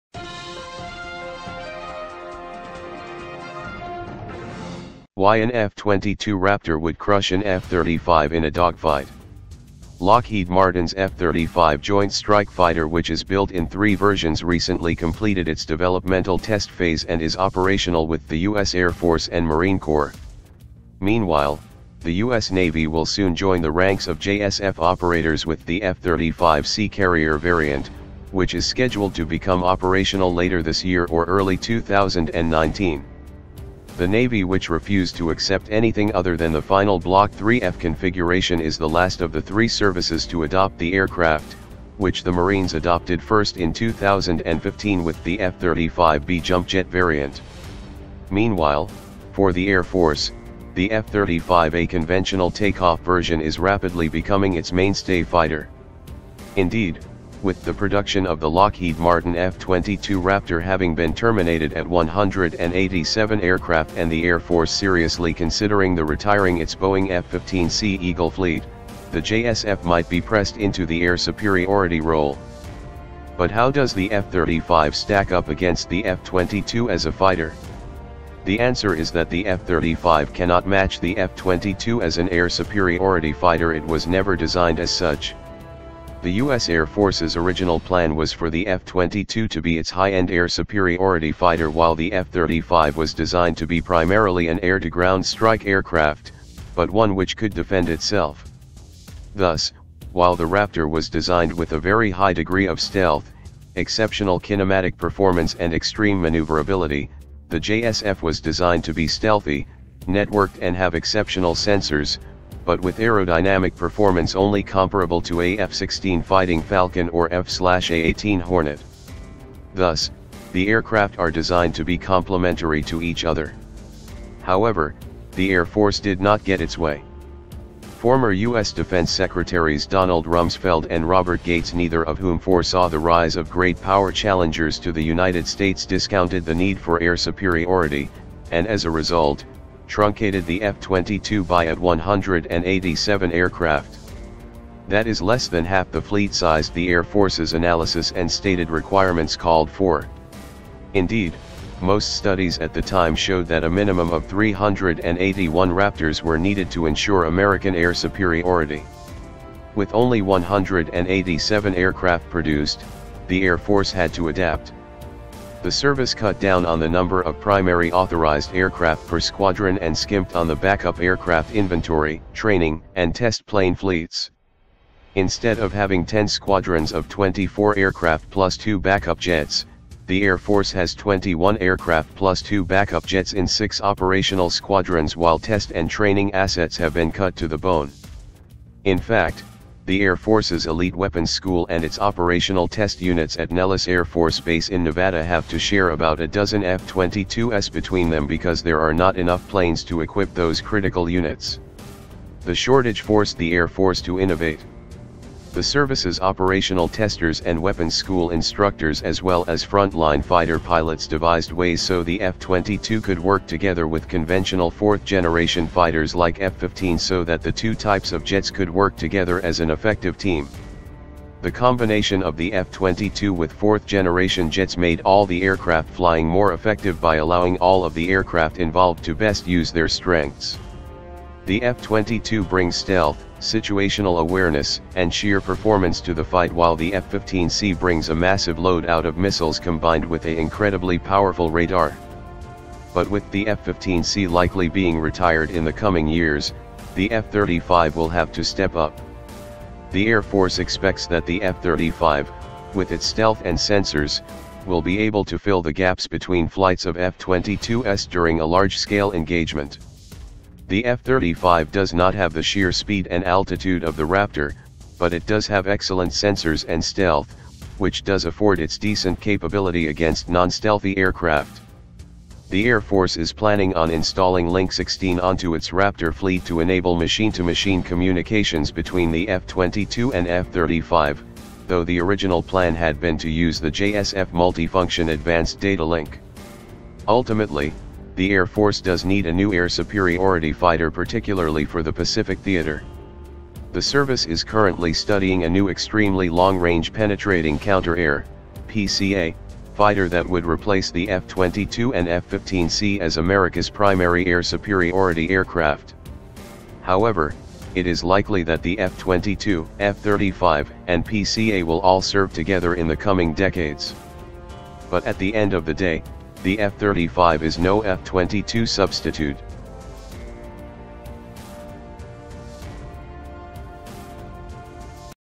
Why an F-22 Raptor Would Crush an F-35 in a 'Dogfight'
Lockheed Martin’s F-35 Joint Strike Fighter—which is built in three versions—recently completed its developmental test phase and is operational with the U.S. Air Force and Marine Corps. Meanwhile, the U.S. Navy will soon join the ranks of JSF operators with the F-35C carrier variant, which is scheduled to become operational later this year or early 2019.

The Navy—which refused to accept anything other than the final Block 3F configuration—is the last of the three services to adopt the aircraft, which the Marines adopted first in 2015 with the F-35B jump-jet variant. Meanwhile, for the Air Force, the F-35A conventional takeoff version is rapidly becoming its mainstay fighter. Indeed, with the production of the Lockheed Martin F-22 Raptor having been terminated at 187 aircraft and the Air Force seriously considering the retiring its Boeing F-15C Eagle fleet, the JSF might be pressed into the air superiority role. But how does the F-35 stack up against the F-22 as a fighter?